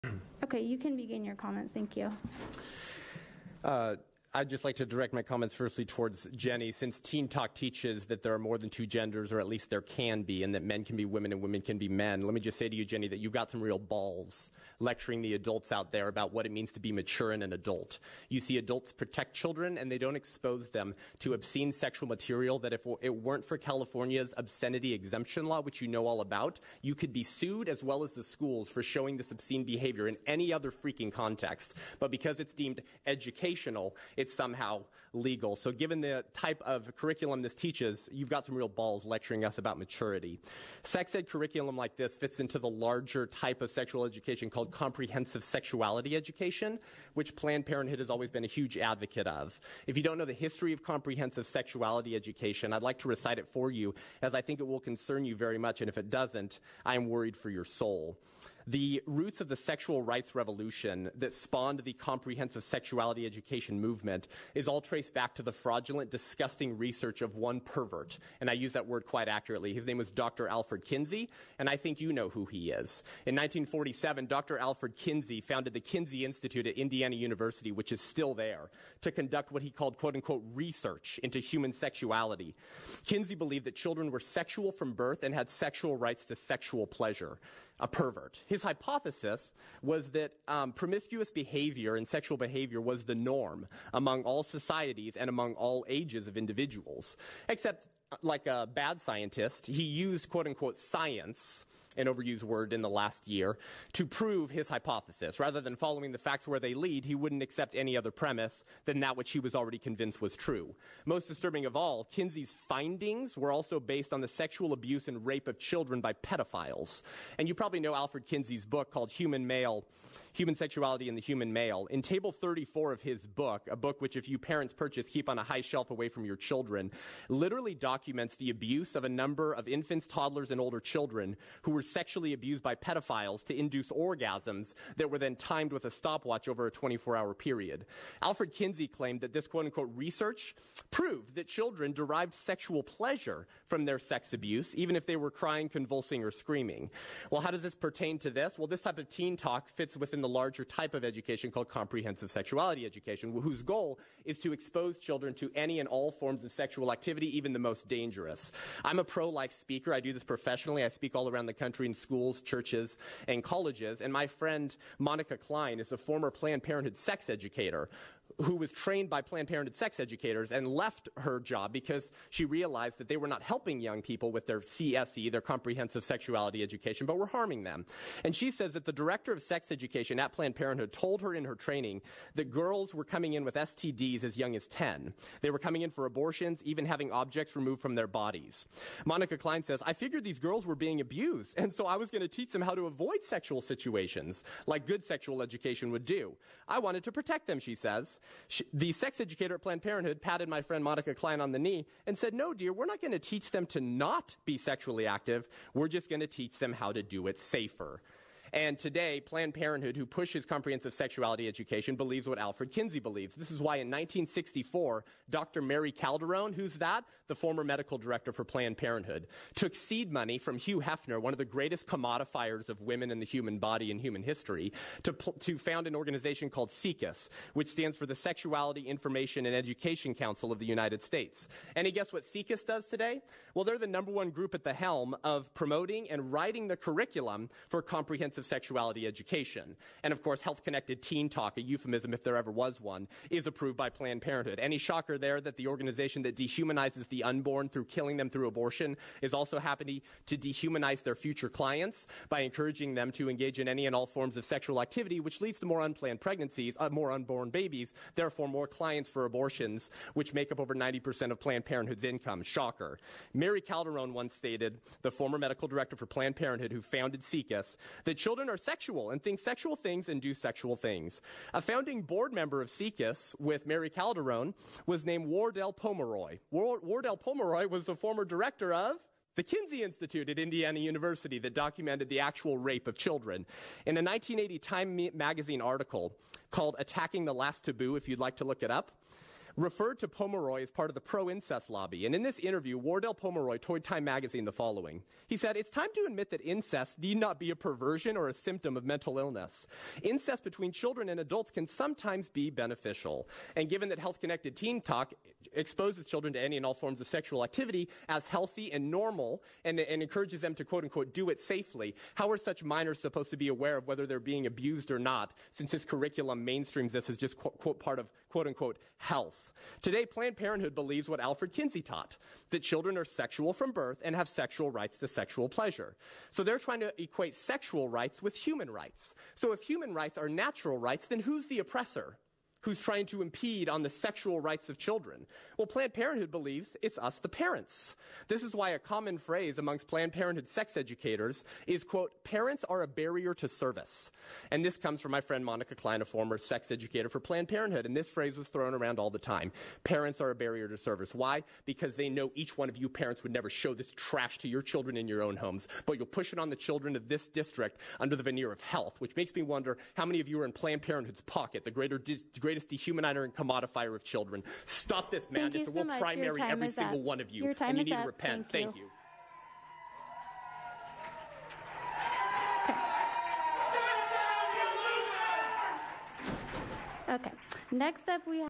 Pro-Life Speaker Exposes School Board's Disgusting "Sex-Ed" Curriculum
Pro-Life speaker Seth Gruber blasts the Conejo Valley Unified School District for pushing Planned Parenthood approved "Health Connected Teen Talk" and exposes the history of Comprehensive Sexuality Education, which exposes minors to any and all forms of sexual activity; even the most dangerous.